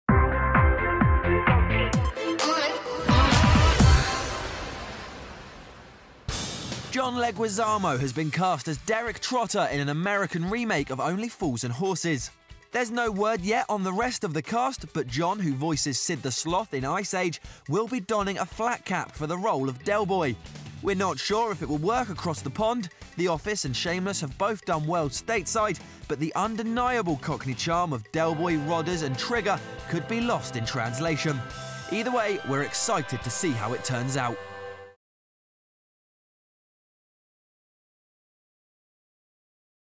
John Leguizamo cast as Del Boy in US Only Fools and Horses
American actor John Leguizamo has been cast as Derek Trotter in a new American version of Only Fools and Horses. Report by Sam Homewood. Test